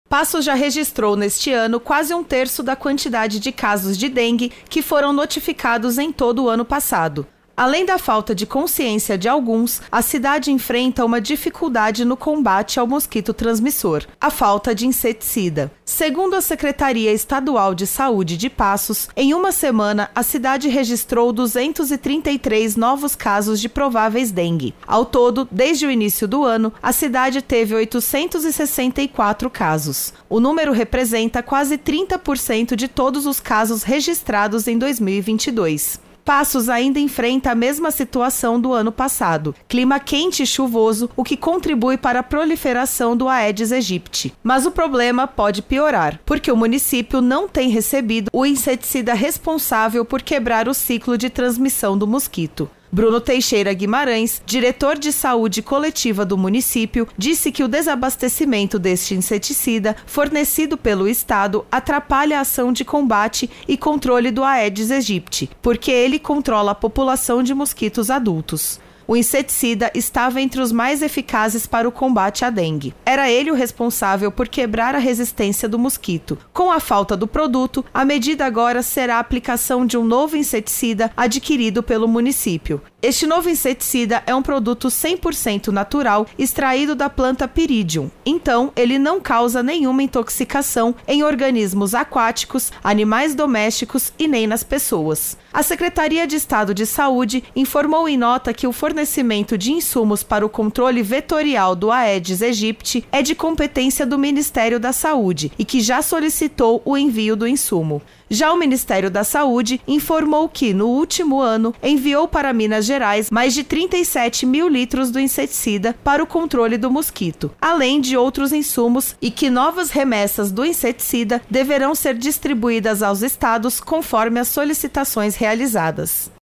FALTA DE INSETICIDA PREJUDICA AUTORIDADES DE SAÚDE NO COMBATE À PROLIFERAÇÃO DA DENGUE EM PASSOS
| REGIÃO |

EM PASSOS, A FALTA DE INSETICIDA TEM PREJUDICADO OS AGENTES DE SAÚDE NO COMBATE À PROLIFERAÇÃO DA DENGUE.